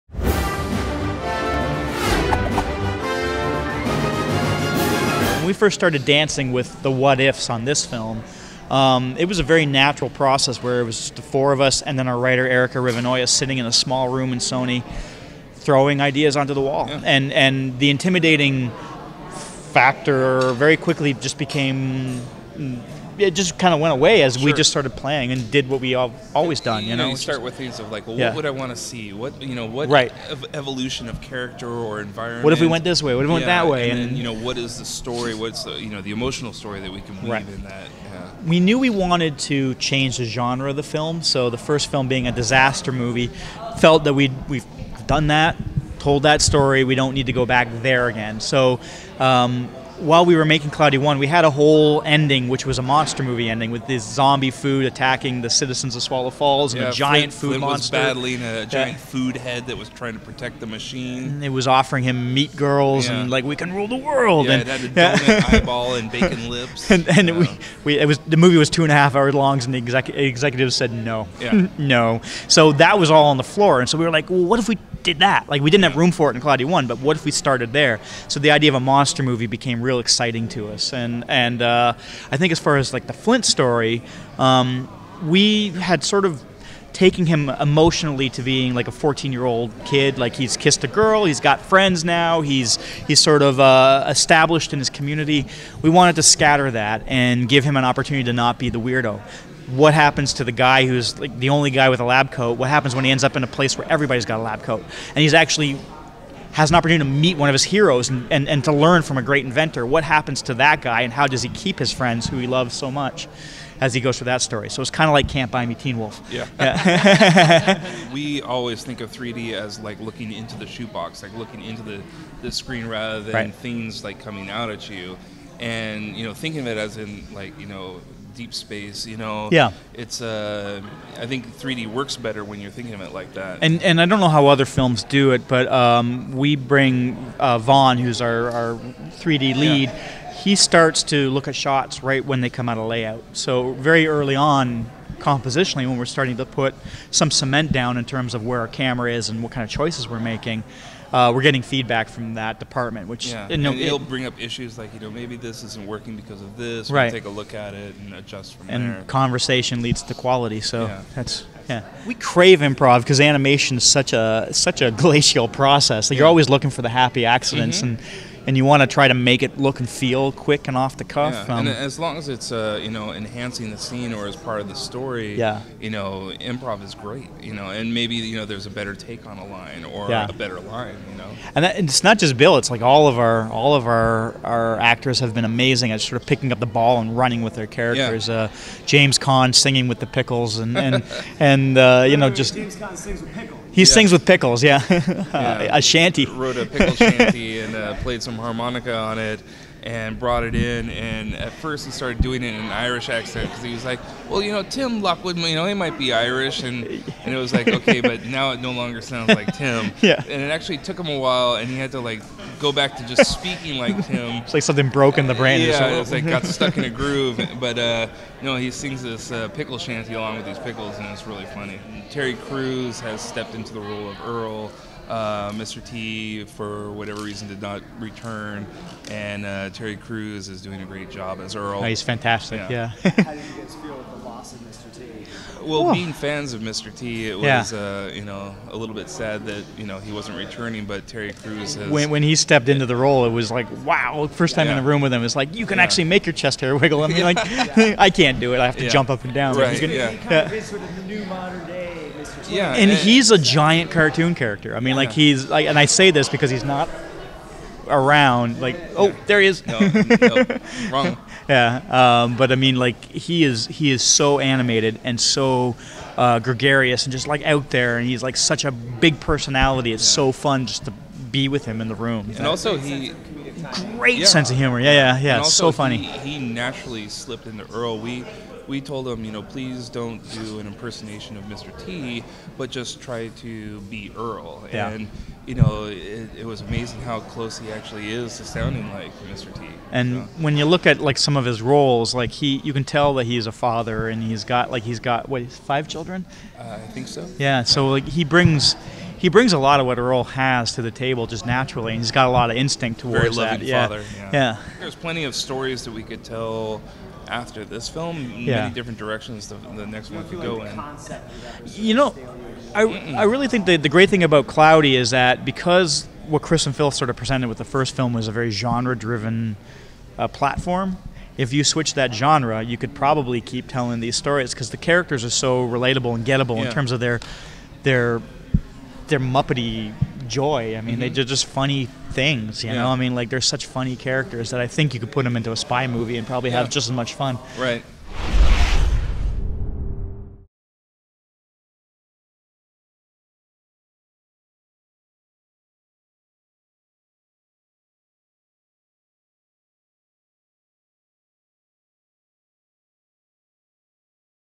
SDCC 2013: Directors Kris Pearn and Cody Cameron Talk 'Cloudy with a Chance of Meatballs 2'
IAR managing editor Jami Philbrick speaks exclusively with co-directors Kris Pearn and Cody Cameron at San Diego Comic-Con International 2013 about their upcoming sequel 'Cloudy with a Chance of Meatballs 2,' which opens in theater son September 27th.

Video Editor: Jami Philbrick

IAR Managing Editor: Jami Philbrick